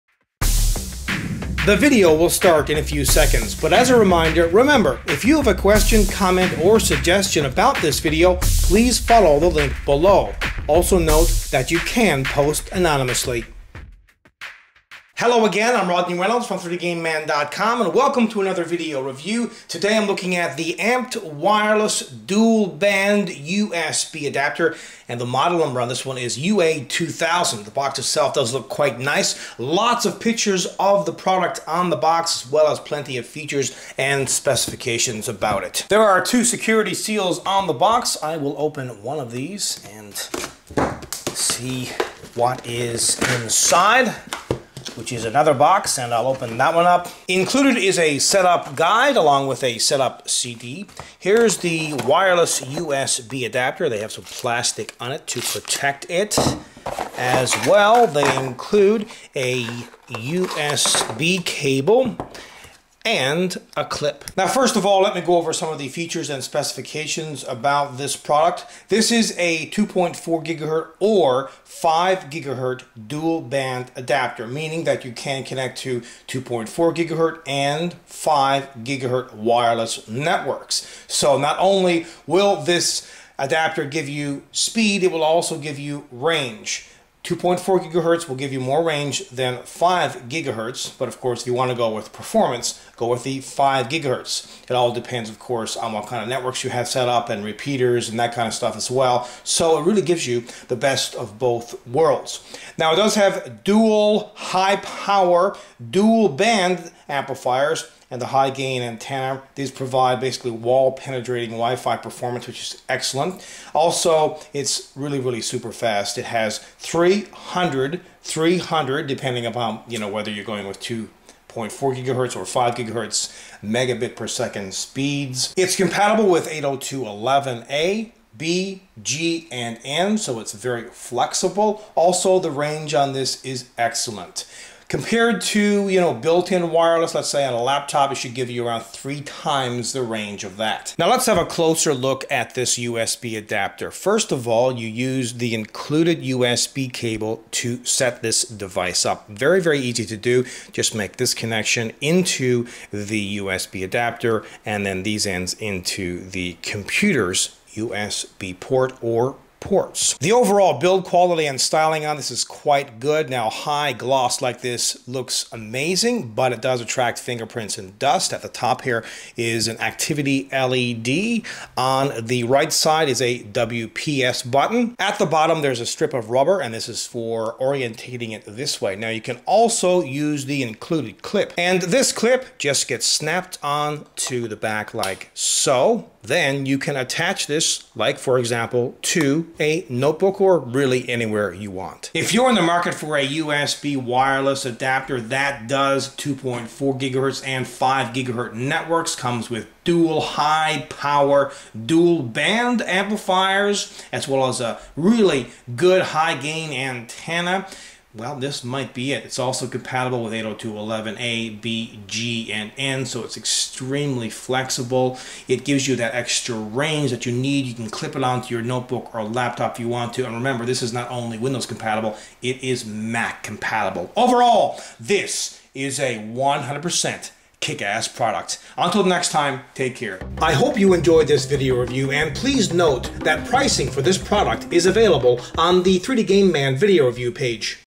#1357 - Amped Wireless UA2000 Dual Band USB Adapter Video Review
The Amped Wireless UA2000 Dual Band USB Adapter is one of the best USB wireless solutions out there. It's capable of 2.4GHz & 5GHz, 802.11a/b/g/n, has dual amplifiers, high gain antenna & the latest security. It also can be clipped onto a notebook for easy transport. This is a small, but powerful wireless adapter which will extend your range by about 3x.